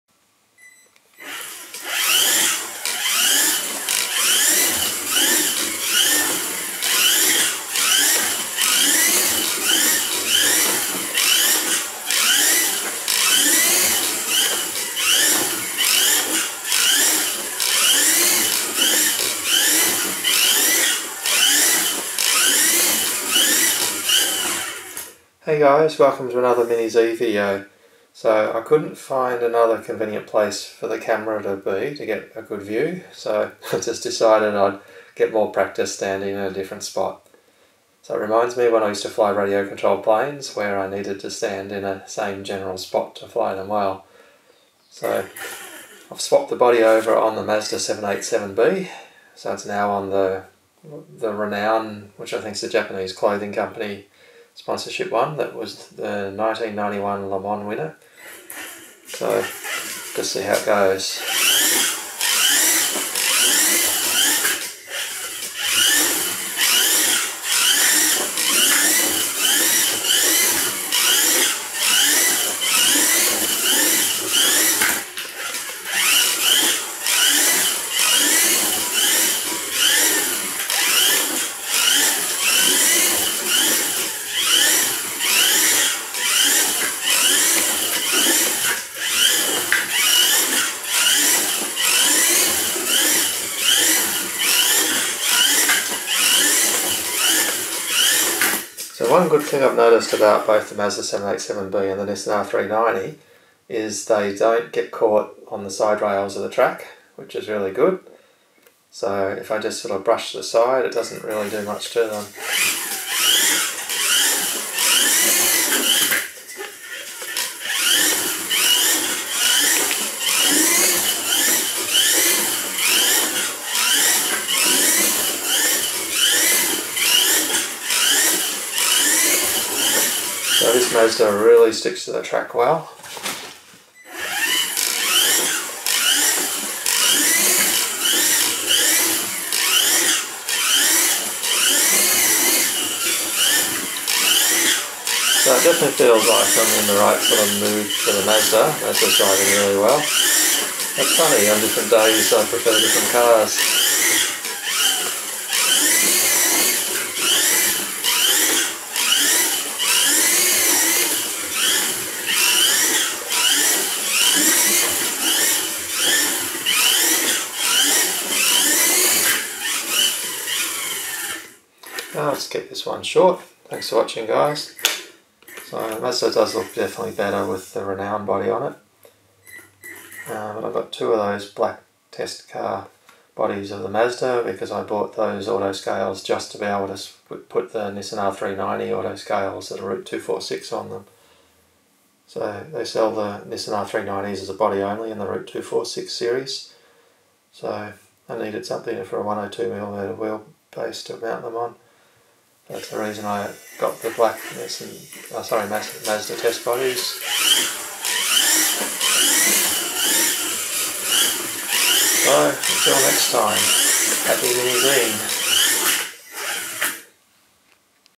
Kyosho Mini-Z Mazda 787B - How Good is This 102mm LM Car ?!?!?!?!?!
I swapped my Mazda 787B black test car body over to a red and green Renown body; the car that won the 1991 Le Mans 24 hour race.  The Mazda 787B is easily one of my absolute favourite Mini-Zs to drive on my track.  I seem to get my most consistent laps with this car, most likely due to how easy it is to drive :).